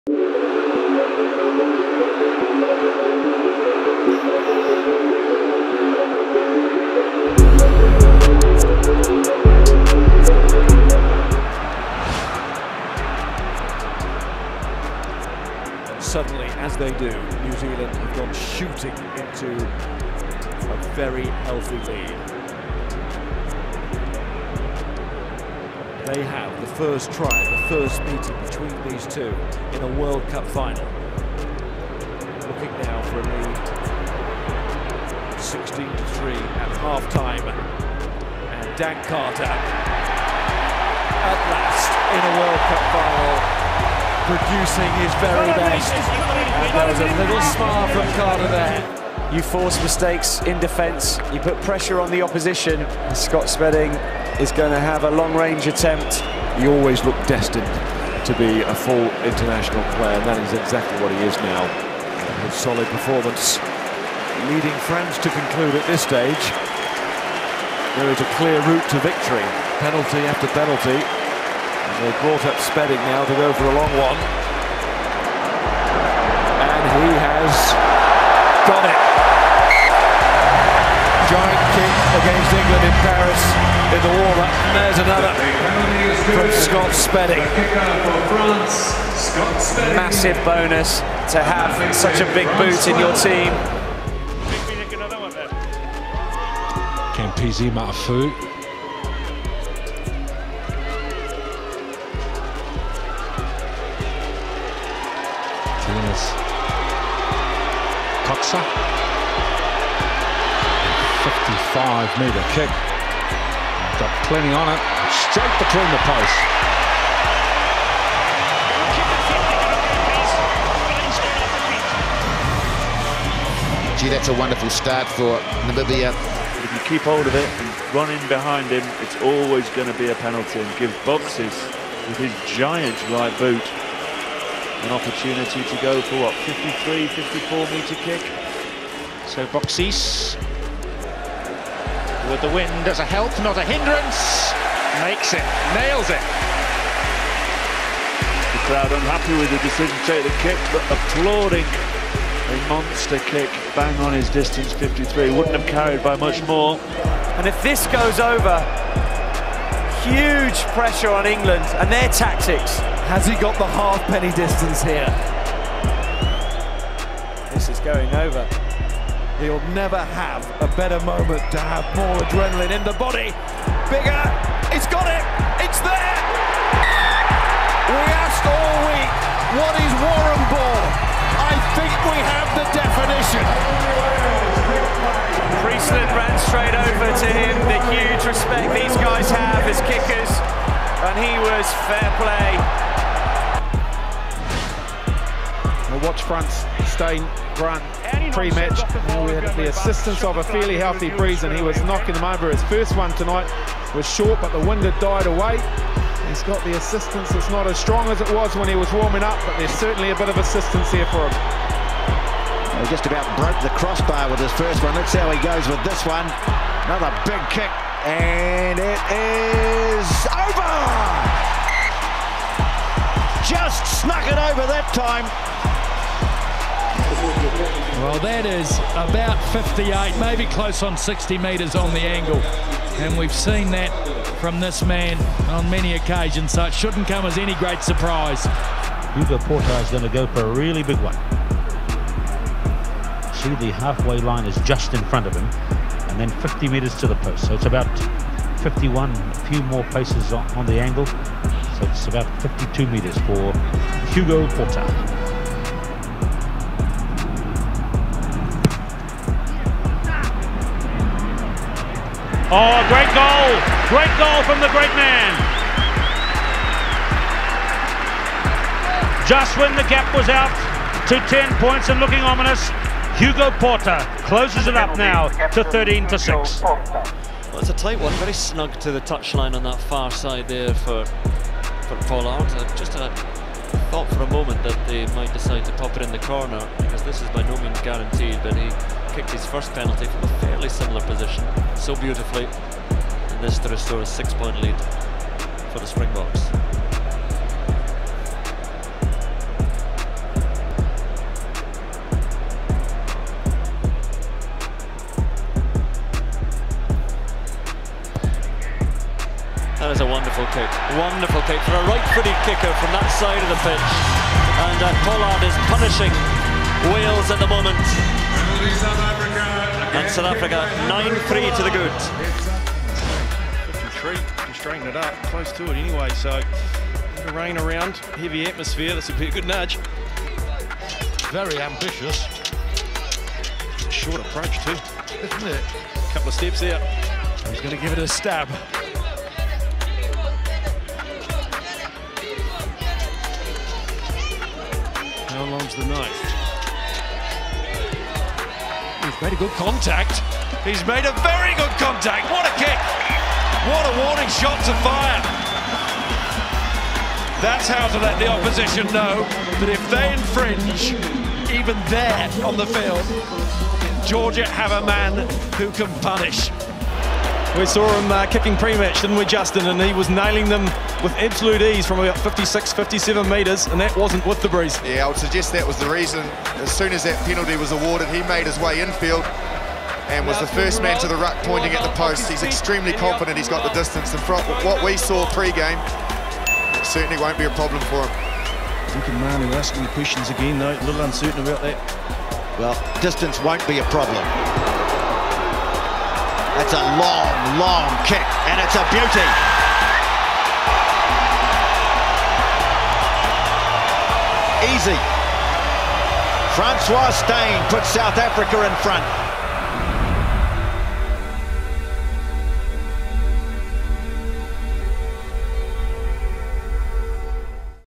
AMAZING and NAIL BITING World Cup Conversions!
There's not quite like the pressure of nail biting conversions especially in a World Cup. Here we count down the most amazing kicks in the history of the competition. Featured in this video: Dan Carter (New Zealand), Dan Biggar (Wales) and Francois Steyn (South Africa)